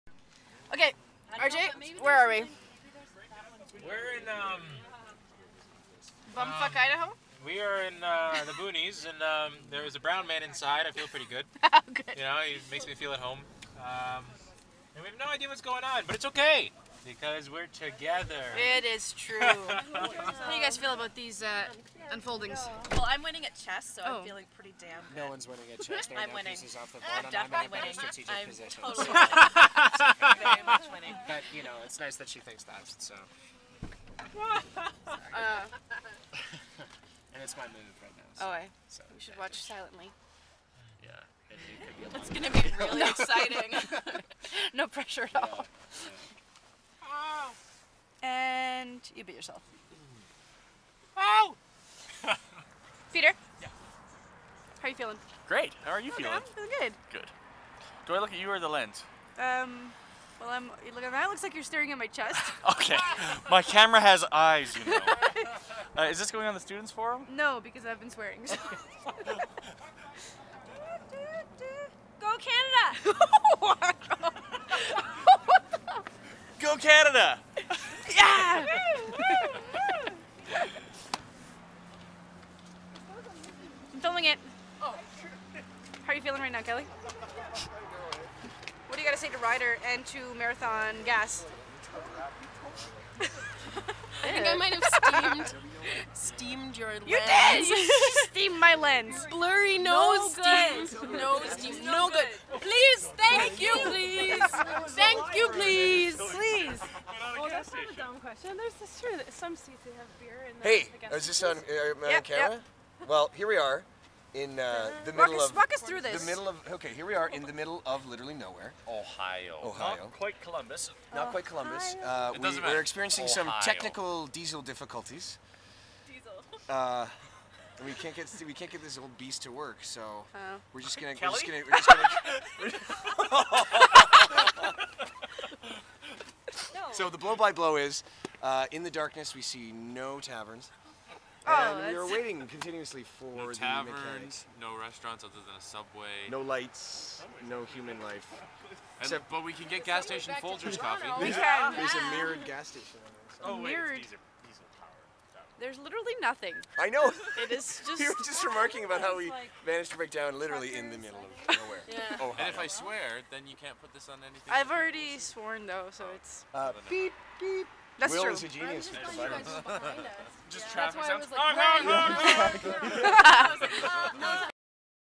Ryder Fail/CTP Tour Day 1
On our way to Bardstown, Kentucky...our Ryder truck breaks down in the MIDDLE OF NOWHERE...aka Dry County USA.